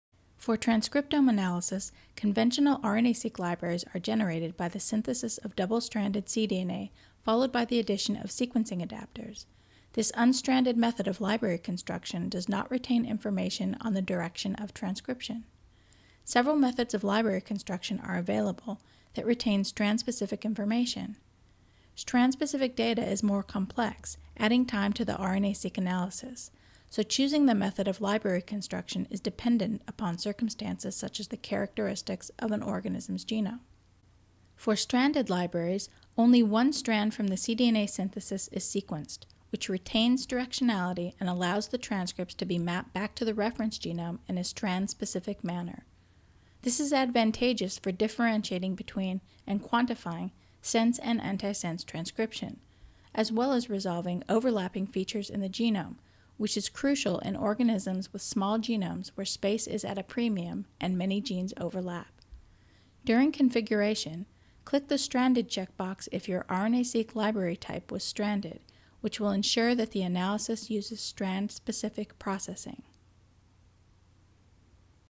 Stranded Libraries
RNA-seq analysis configuration on the Maverix Analytic Platform: Stranded vs unstranded libraries.